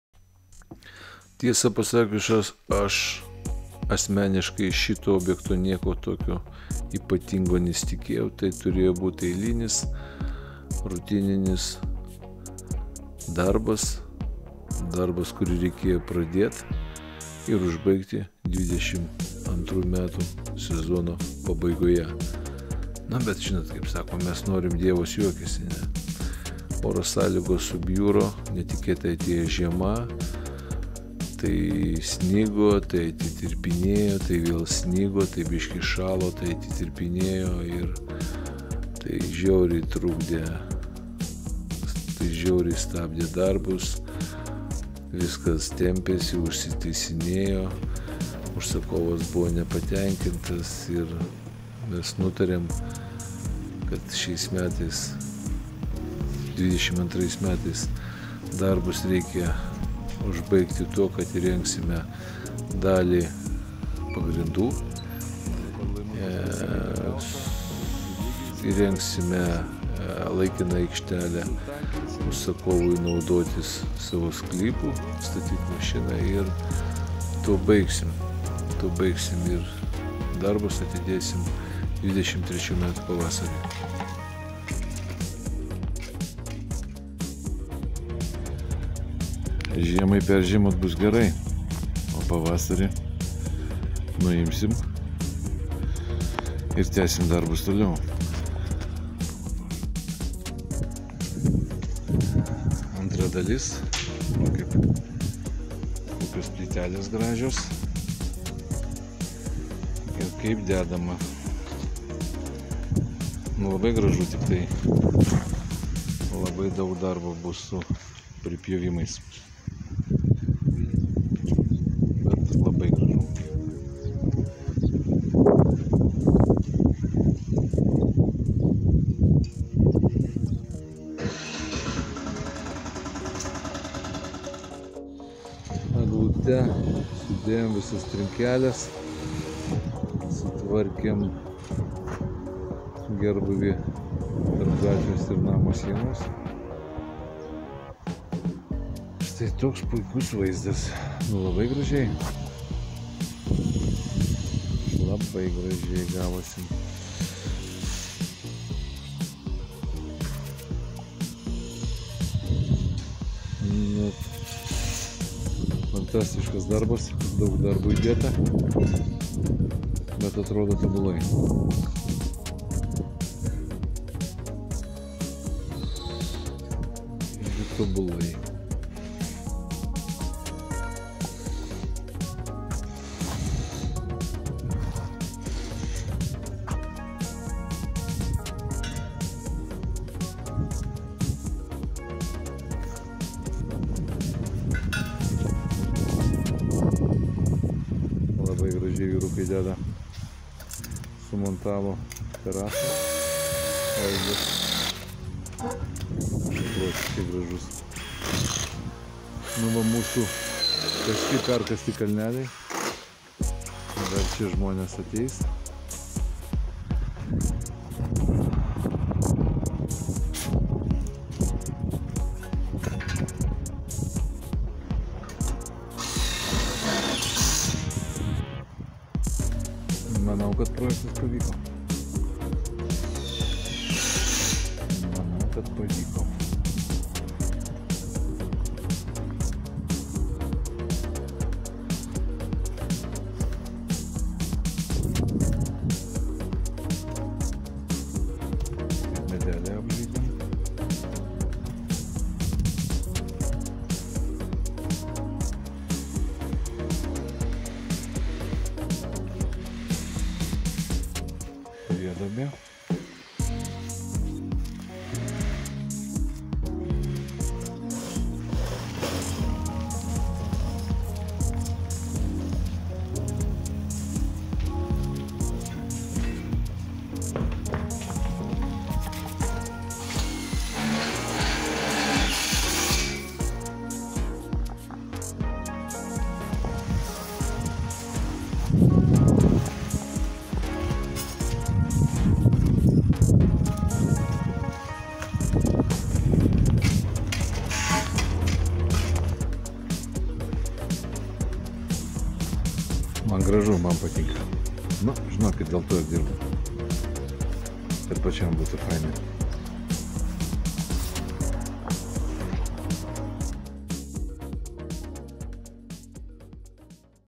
Net nesitikėjau,kad vaizdas bus toks geras!
Trinkelių dangos įrengimas.Trinkelės suklotos eglute,vaizdas įspūdingas